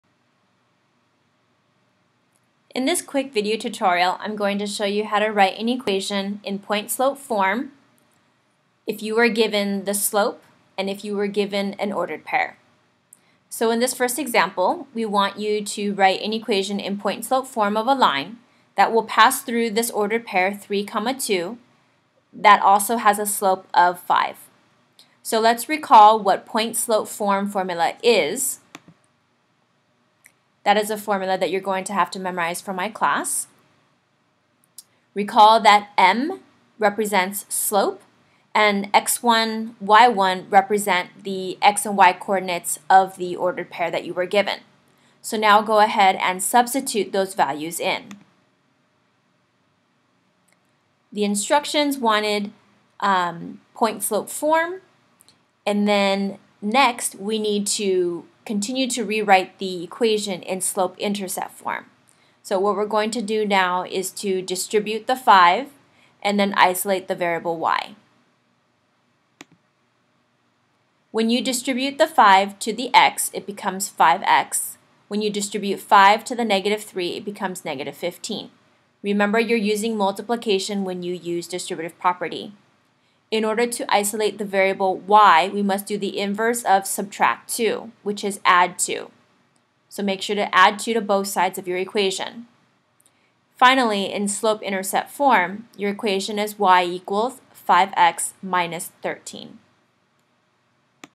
Writing an Equation in Point Slope Form
After watching this video tutorial, you should be able to do the following:
(1) Write an equation in point slope form of the line that passes through a specific ordered pair with a specific slope.